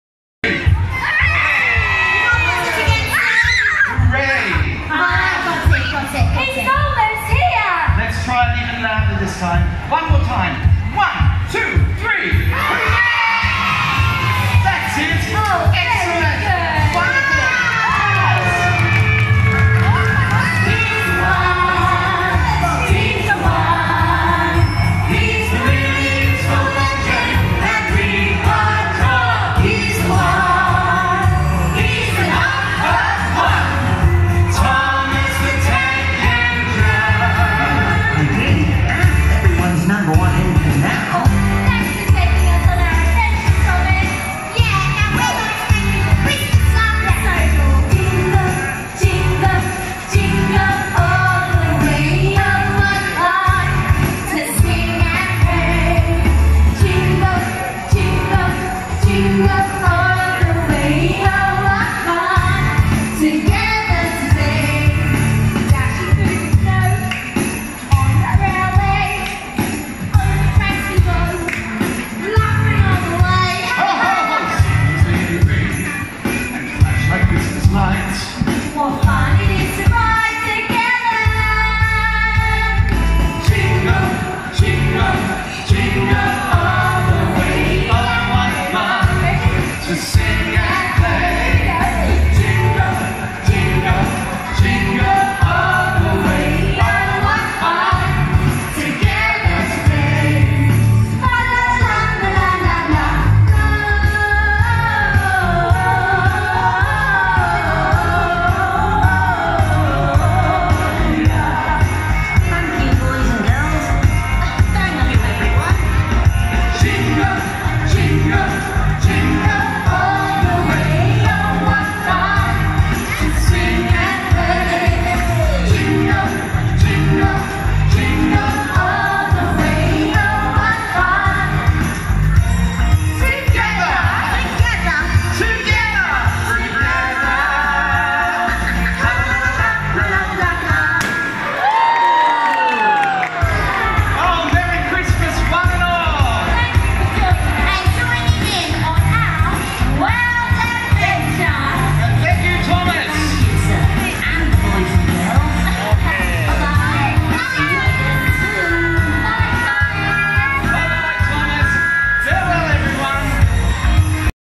Thomas and friends show at Nex Mall in Singapore 1
Thomas train and friends show at Nex Mall in Singapore